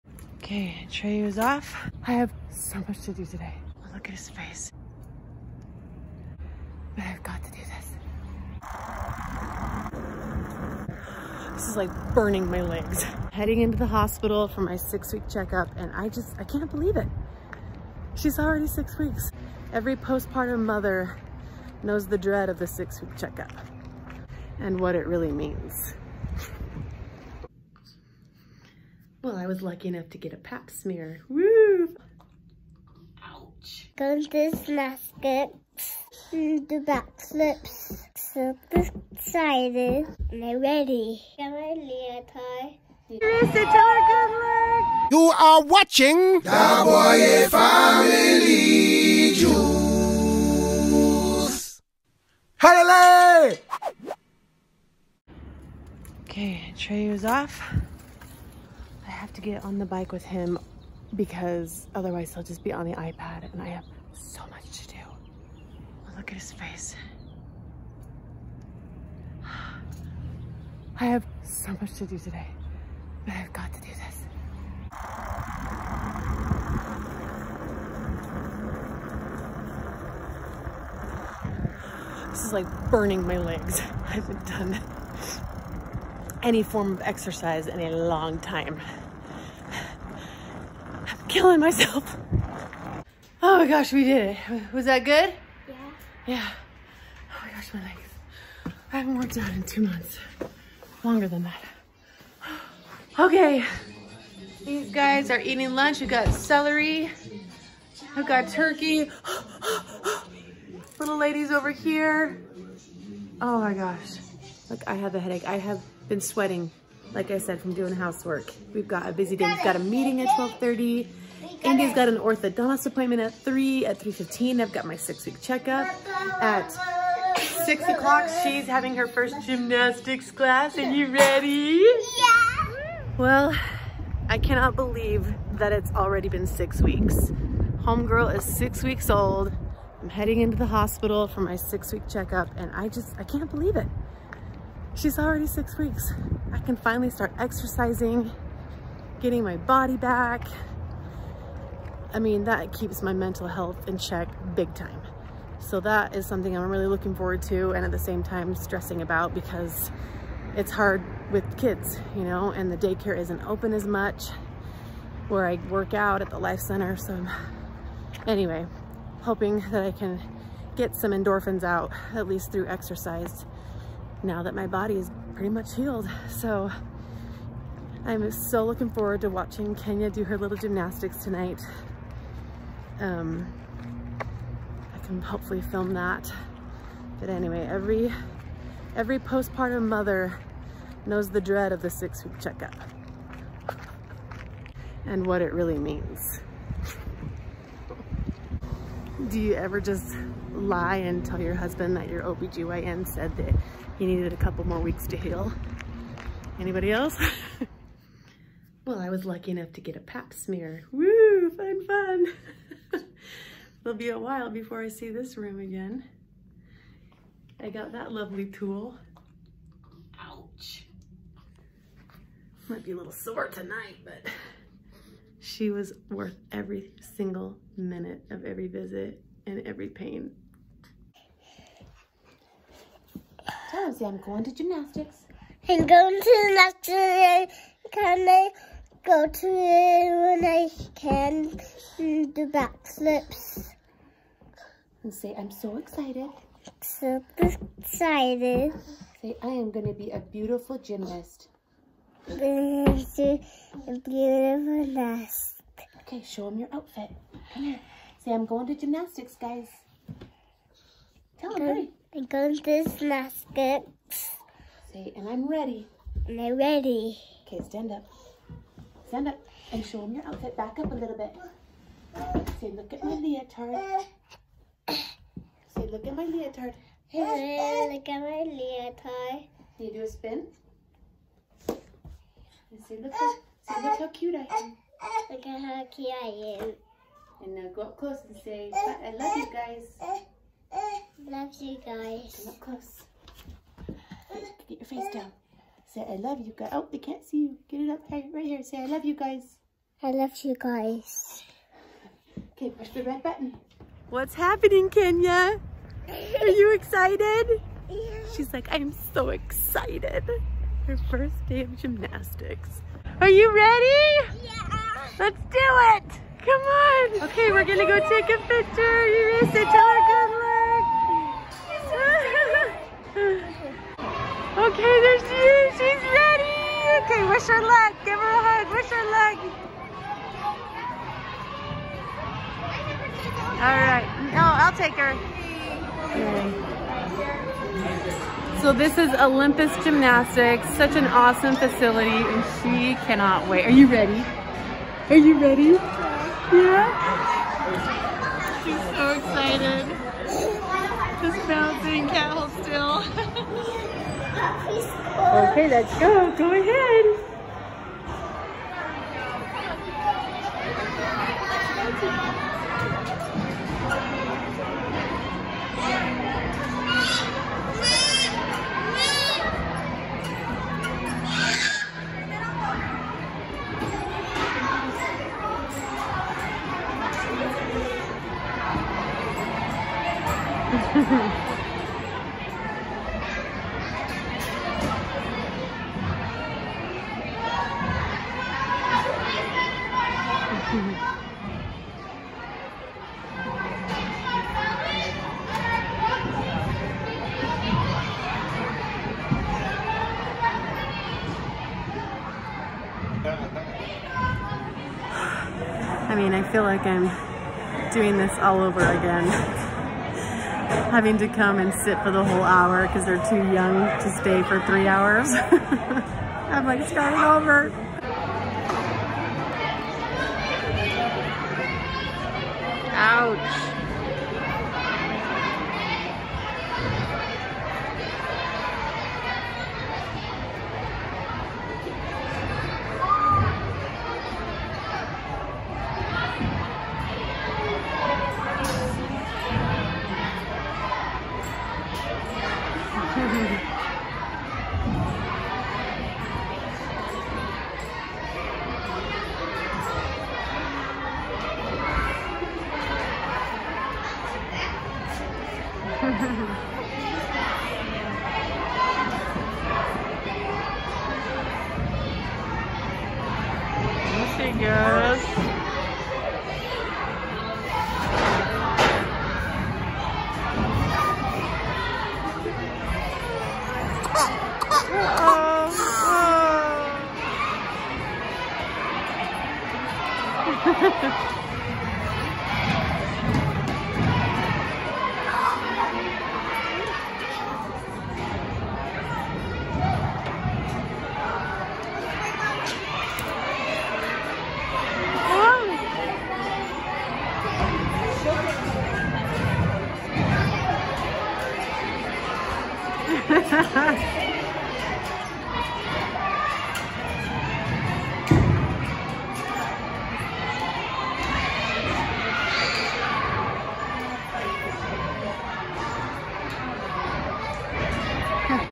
KENIA’S FIRST GYMNASTICS CLASS!
She was so excited she could hardly stand it!

(remember the j in the middle)